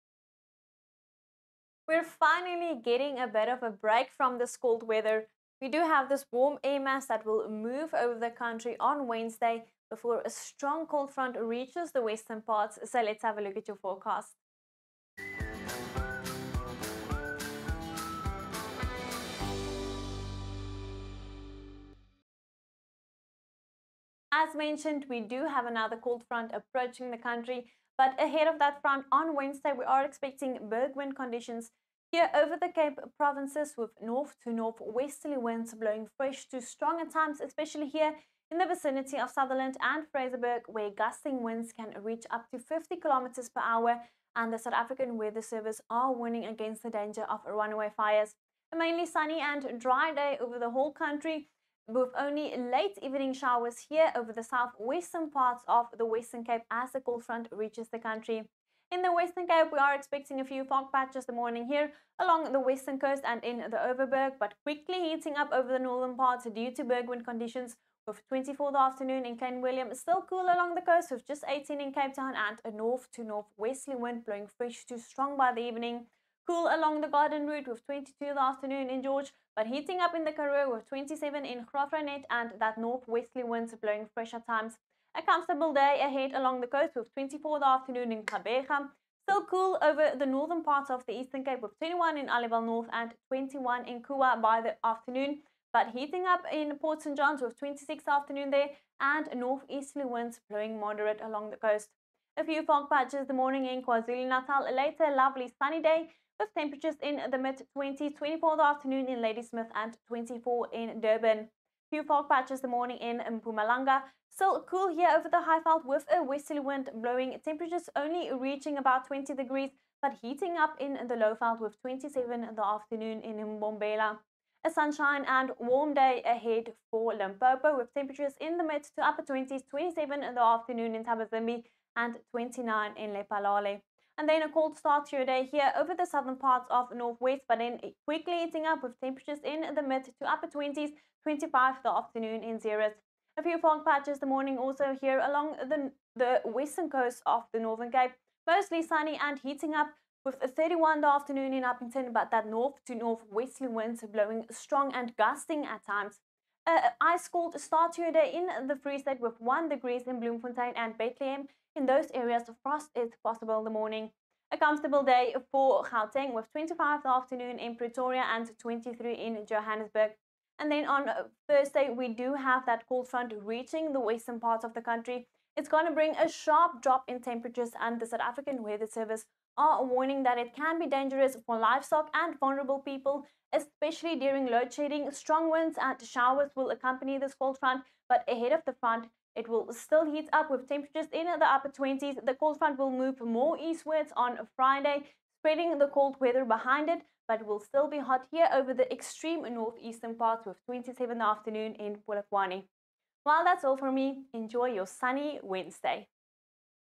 24 May 2023 | Vox Weather Forecast
⚠️NO SEVERE WEATHER ALERTS issued by SAWS ⚠️

🔥FIRE DANGER WARNINGS🔥
Extremely high fire danger conditions are expected over the Dawid Kruiper, Kai !Garib and !Kheis Local Municipalities of the Northern Cape.

🥶⚠️ ADVISORIES by SA Weather Service ⚠️🥶
A well-defined cold front with a potent upper air support is expected to make landfall on Thursday (25/05/2023), dropping the daytime temperatures significantly on Thursday into Friday over the Western Cape extending to the Eastern Cape. Maximum temperatures may be below 10C in places over the southern high-lying areas of the Namakwa region of the Northern Cape and over the interior of the Western Cape.

General wet and windy conditions will accompany the cold front. There is a low likelihood that heavy (30-40mm) rain is possible over the mountainous areas of the Cape Metropole and southern parts of the Cape Winelands.